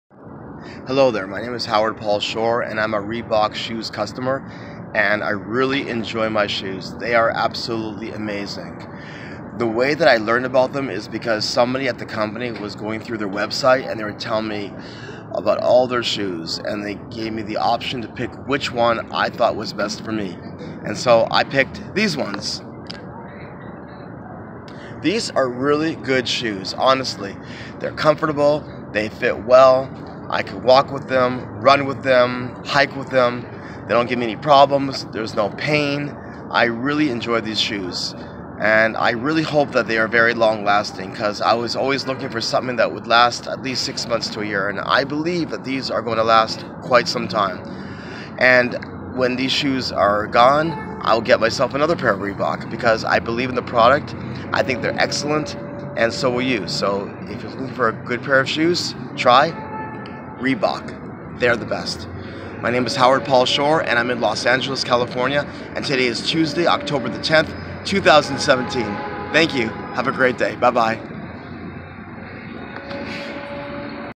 Reebok Shoes, Los Angeles, California
Howard Paul Shore shares his positive experience with his Reebok shoes!  He talks about how he discovered the brand, why he chose his specific pair, and how much he enjoys their comfort and versatility.  If you're looking for a durable and comfortable shoe, Howard highly recommends Reebok.  Have you tried Reebok shoes?